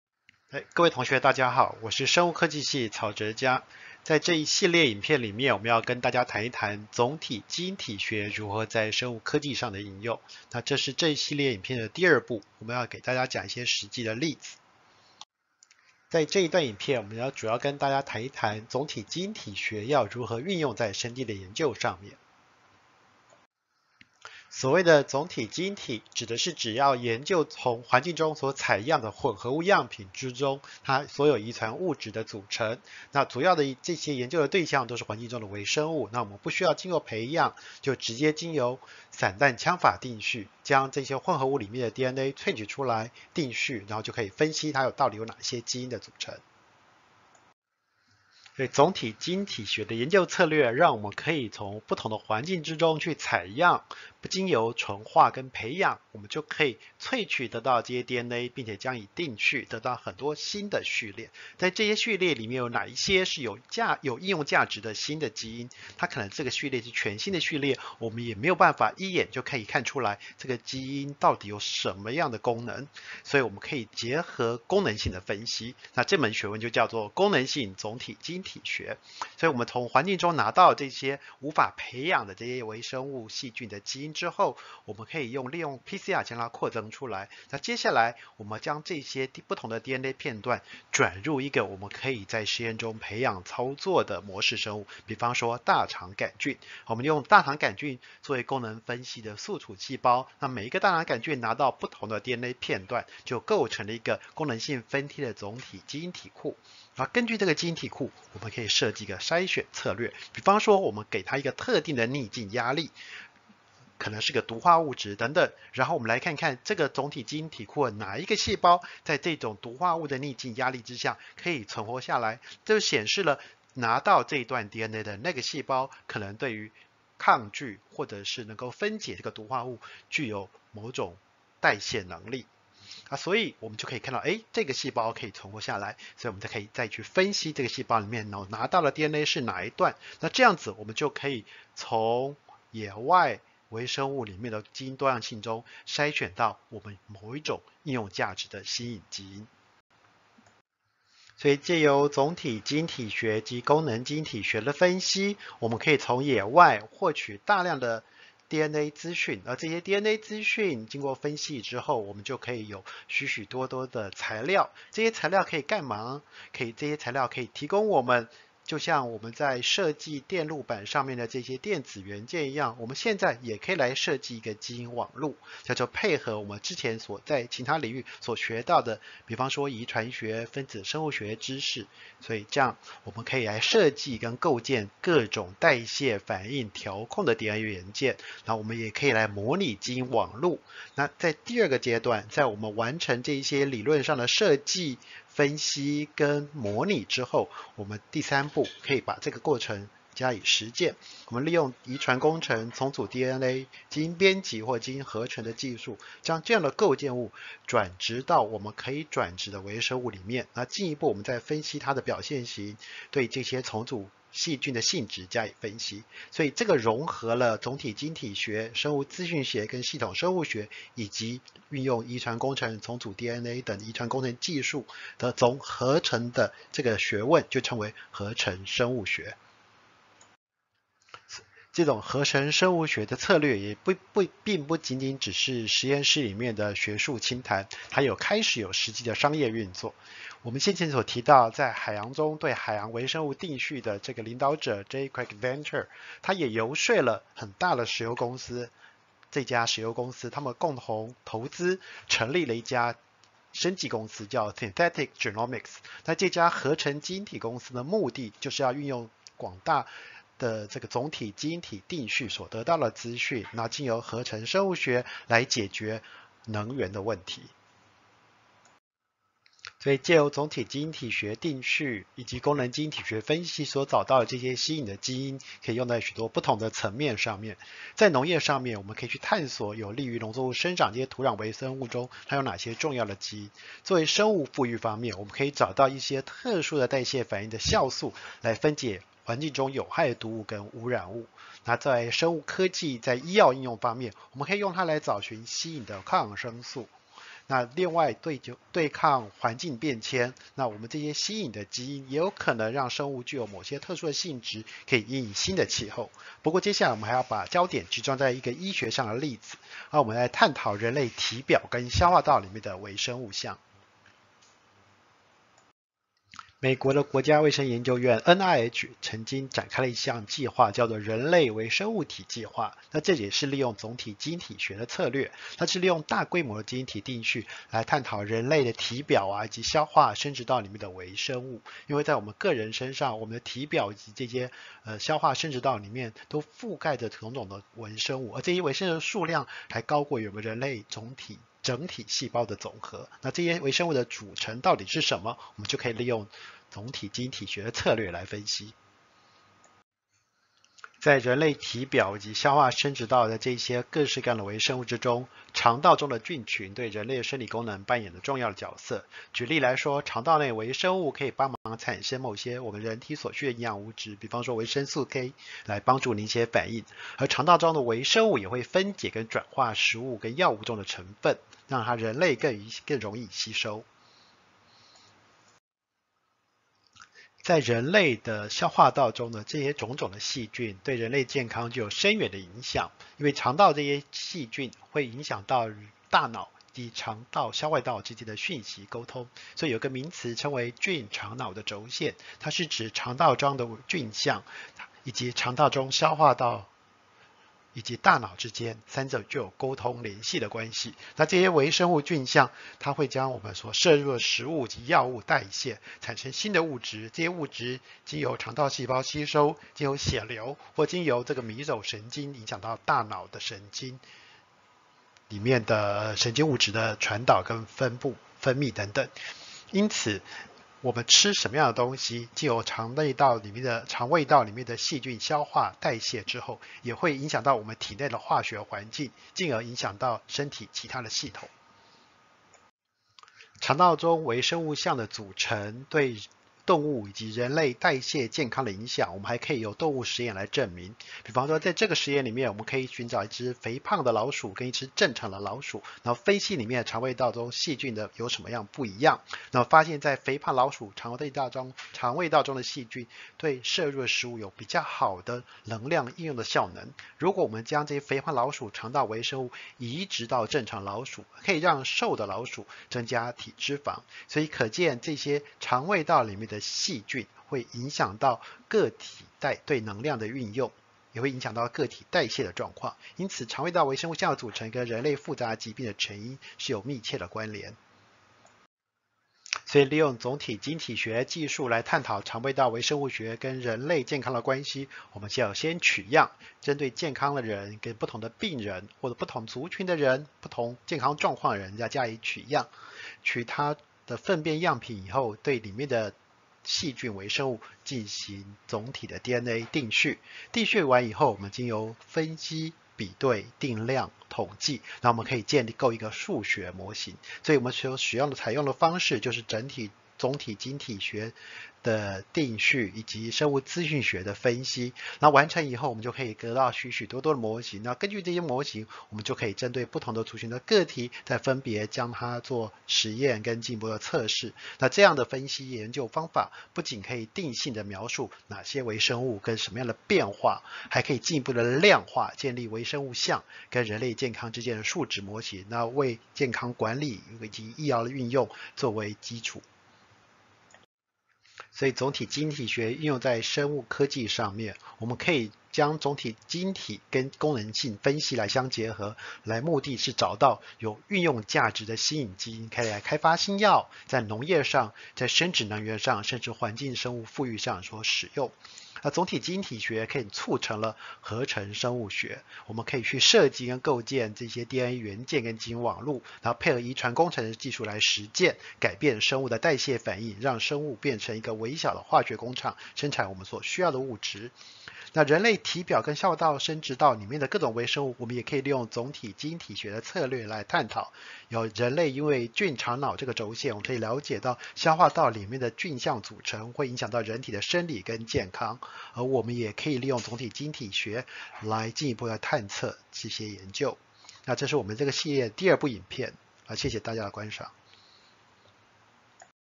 國立臺南大學 核心概念影片【總體基因體學的生技應用 ：構想與實例】
授課教師：國立臺南大學生物科技學系　 曹哲嘉老師